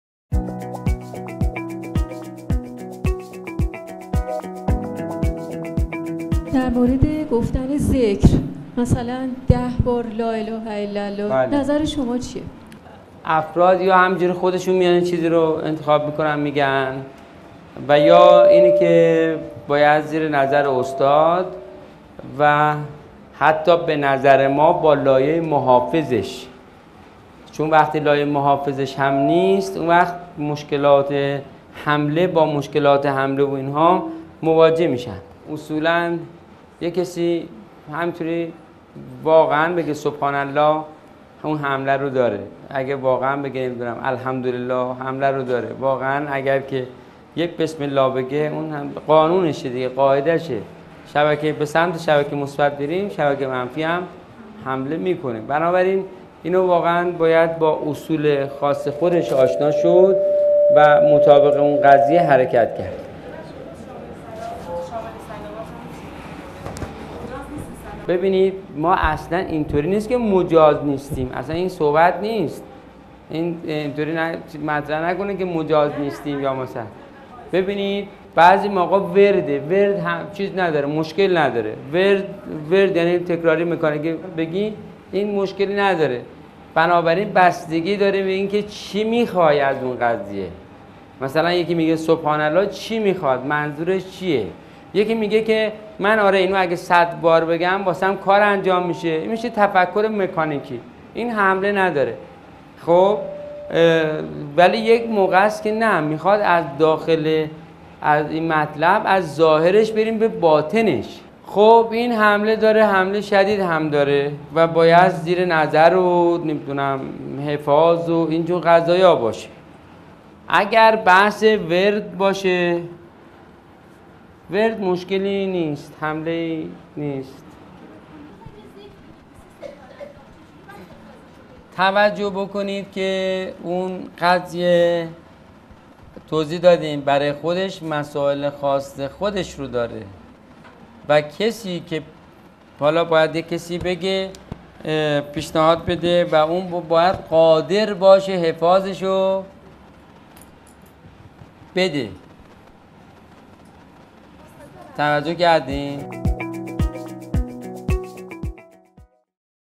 لزوم داشتن لایه محافظ در ذکر
ویدیوهای دروس موضوعی نظام قدیم عرفان کیهانی حلقه (آکادمی طاهری) از سری دروس ارائه شده در دهه هشتاد خورشیدی توسط محمدعلی طاهری می‌باشد. این ویدیو بخش کوتاهی از جلسه پنجم دوره سه نسخه یک نظام قدیم عرفان کیهانی حلقه است. شما می‌توانید بقیه مباحث مطرح شده در این جلسه را در پلی لیست مربوطه دنبال بفرمایید.